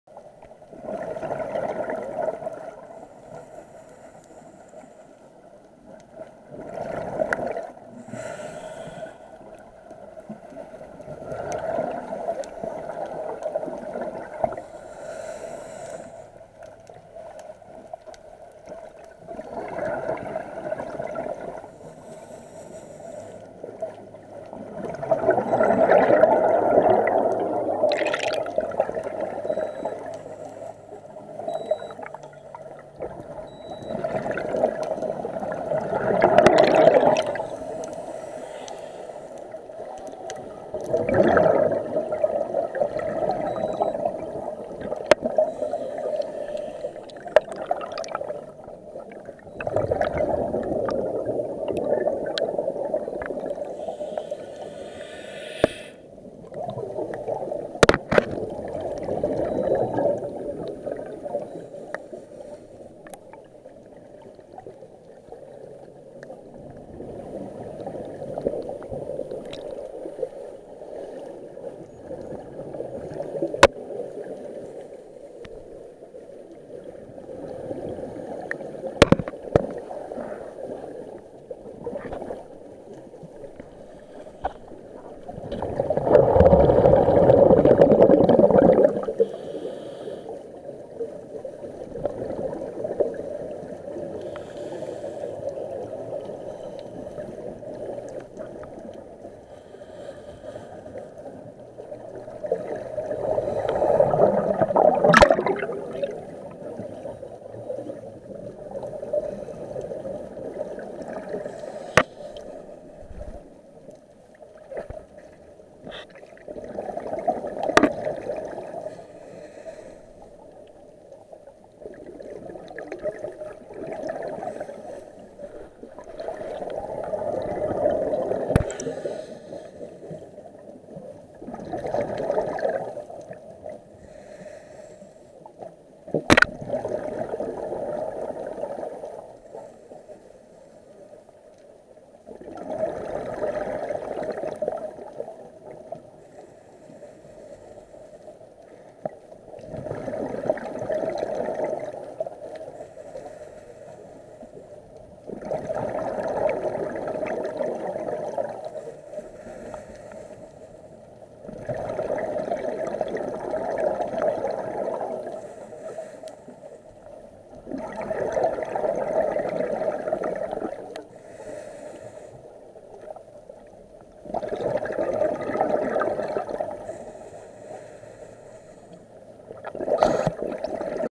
Bahamas: Washing machine dive
Up and down and round and round in the washing machine dive. south of Highbourne Caye, Bahamas